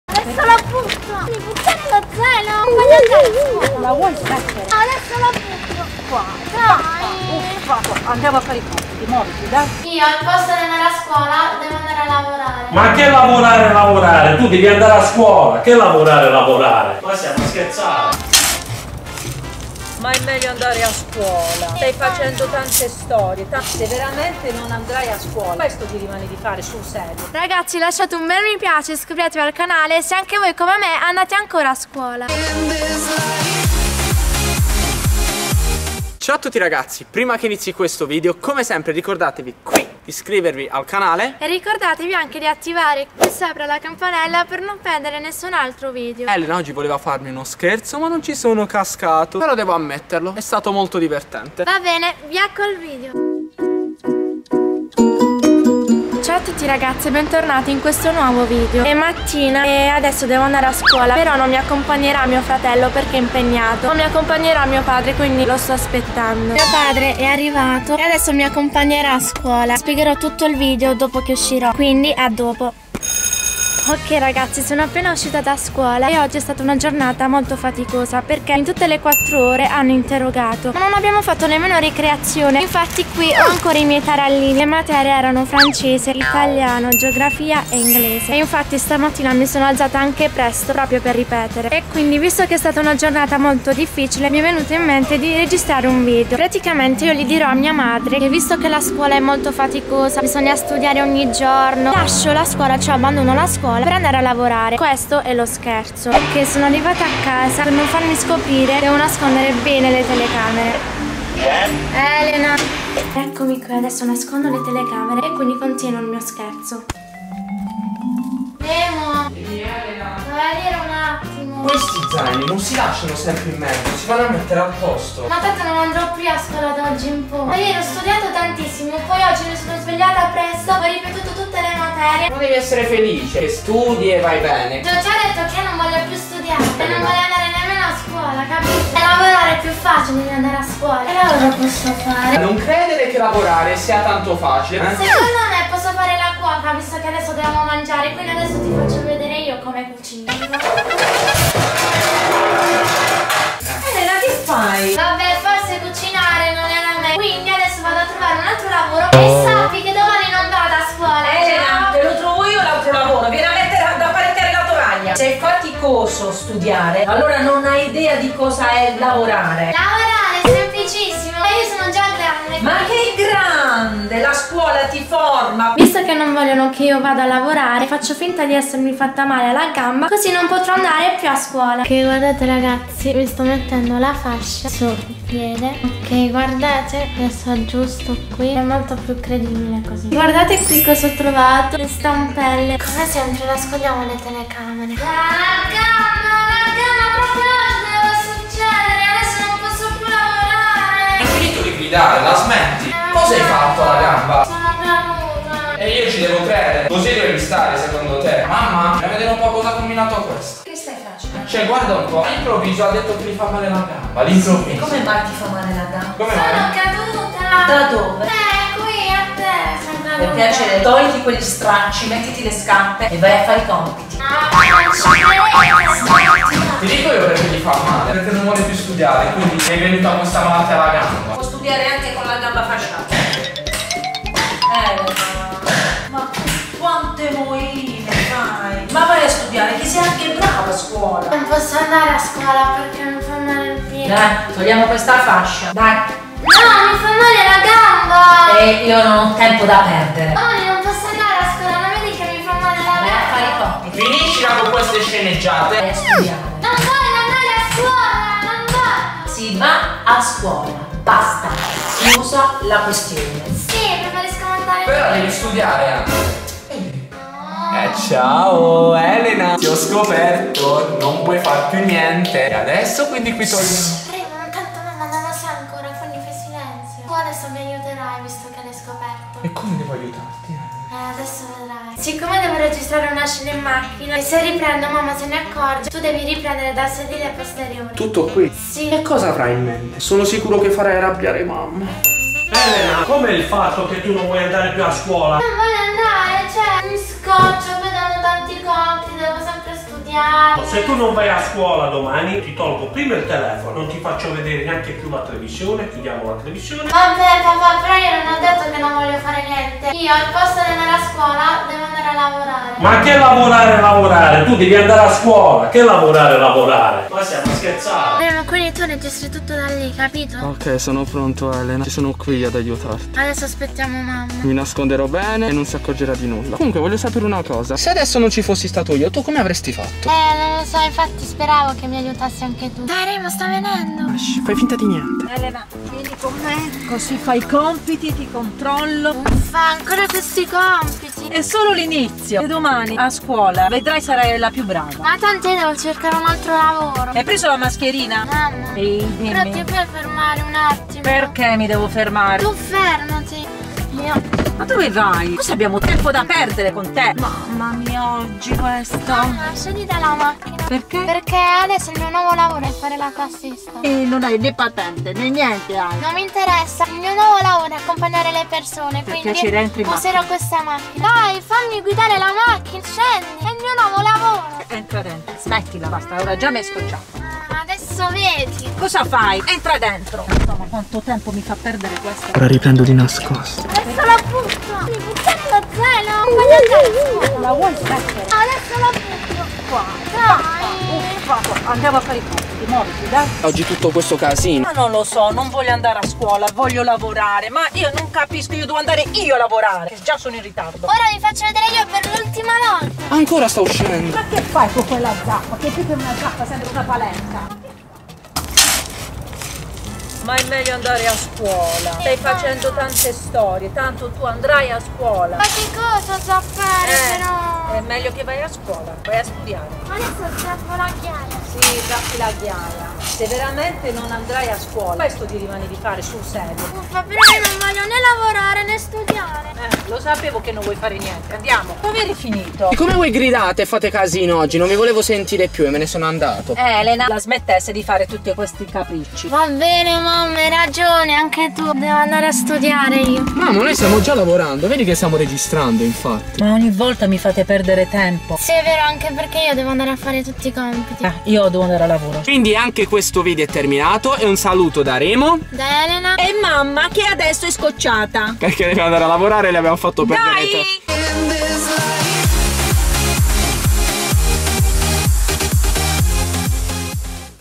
ELENA LASCIA LA SCUOLA A 12 ANNI PER ANDARE A LAVORARE *mamma arrabbiata*
Spero vi sia piaciuto : )